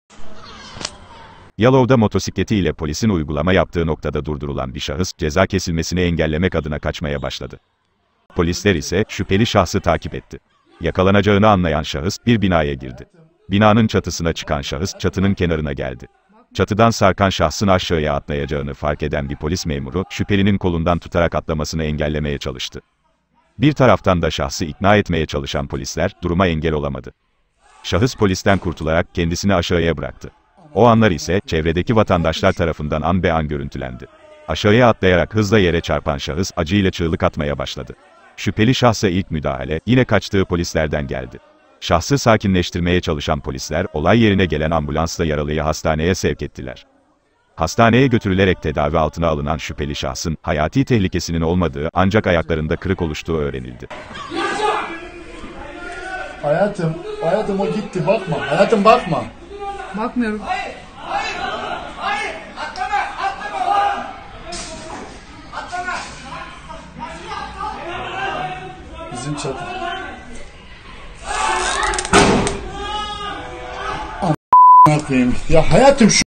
POLİSTEN KAÇAN ŞAHIS ÇATIDAN ATLADI
Yalova’da polisin uygulama yaptığı noktada motosikleti ile durdurulan şahıs ceza yazılmasından kurtulmak için kaçarak çatısına çıktığı binadan polisin tüm ikna çabalarına rağmen atladı.